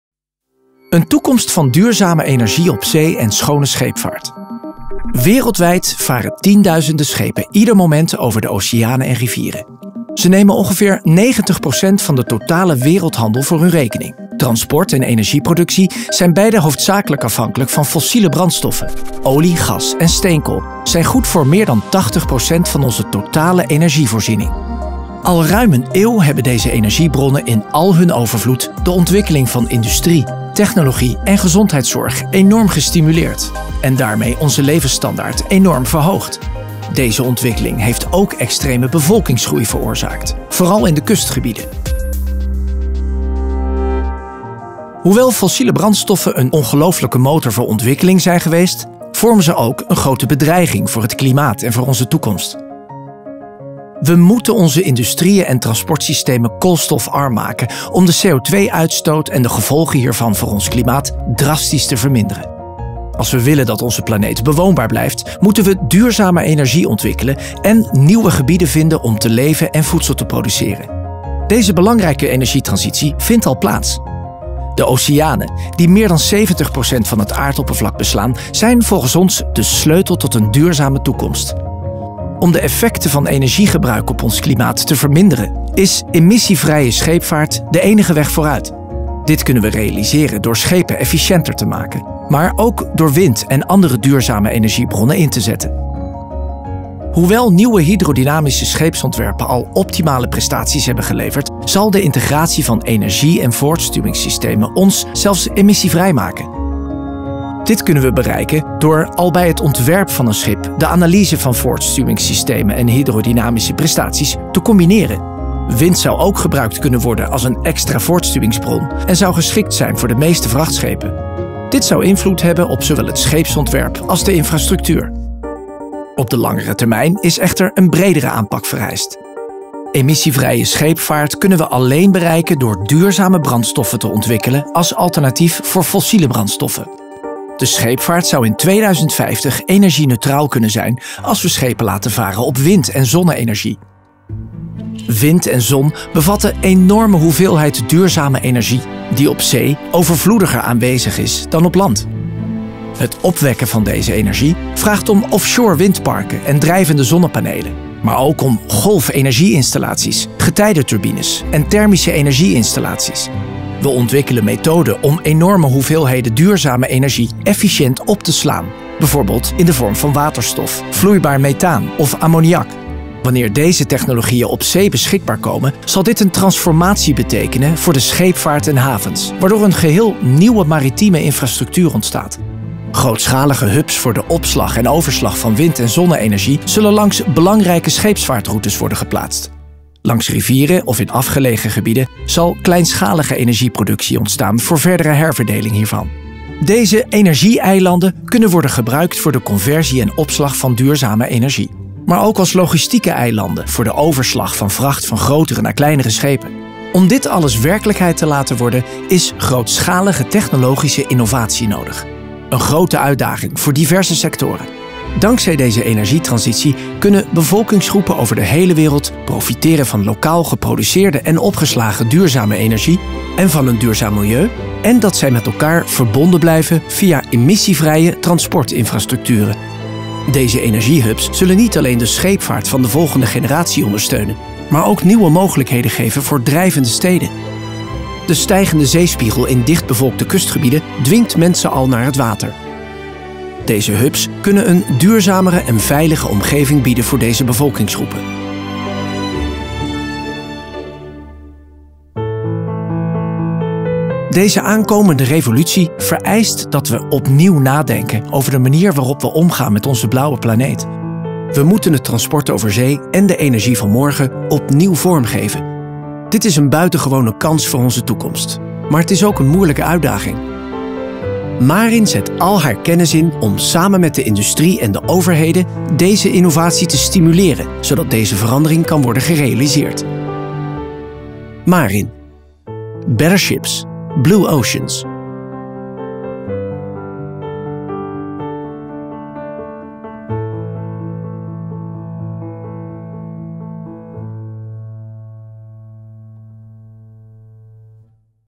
MARIN - Een toekomst van duurzame energie op zee & schone scheepvaart
Better Ships, Blue Oceans - schepen en operaties op zee schoner, slimmer en veiliger maken en bijdragen aan een duurzaam gebruik van de zee. Zo linken we renewable energy aan missieloos varende schepen en drijvende eilanden.